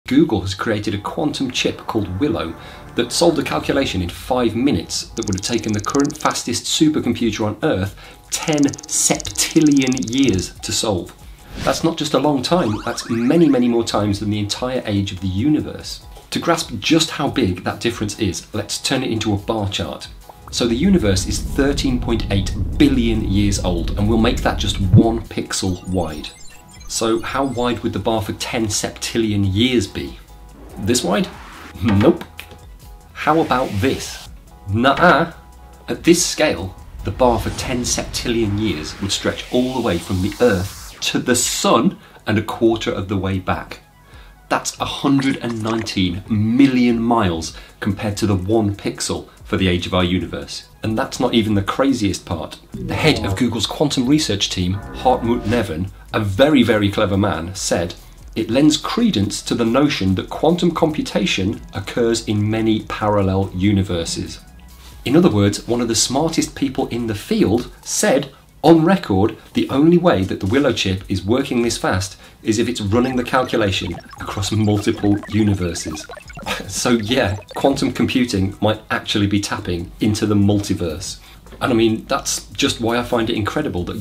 New Google experiment hints the Multiverse is real
Google's new quantum computer chip, Willow, has performed a calculation in 5 minutes that would have taken the fastest supercomputer on Earth 10 Septillion years to solve. Hartmut Neve, the head of Google's Quantum AI division, says to operate this fast, it could well be running the calculation across multiple universes!